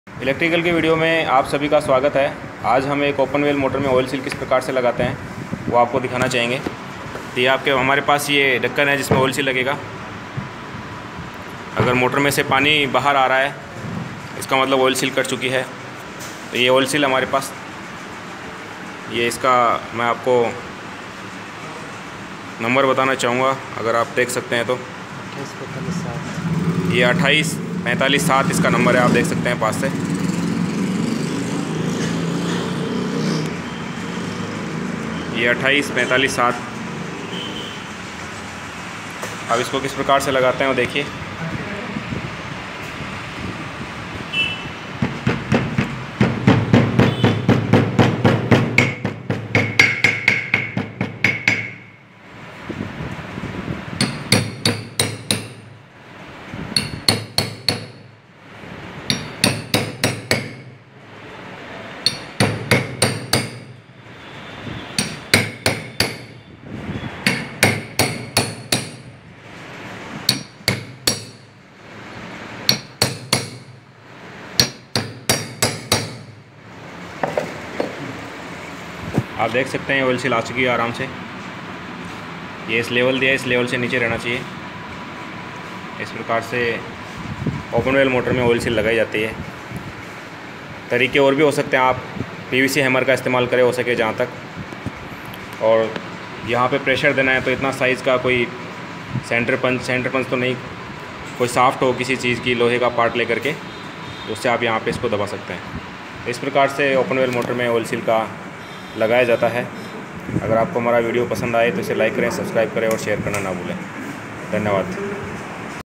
ओपन वेल मोटर में ओइलशील लगाना सीखे।
दोस्तो इस वीडियो के  माध्यम से आप ओएन वेल मोटर में ओइलशील लगाना सिख सकते है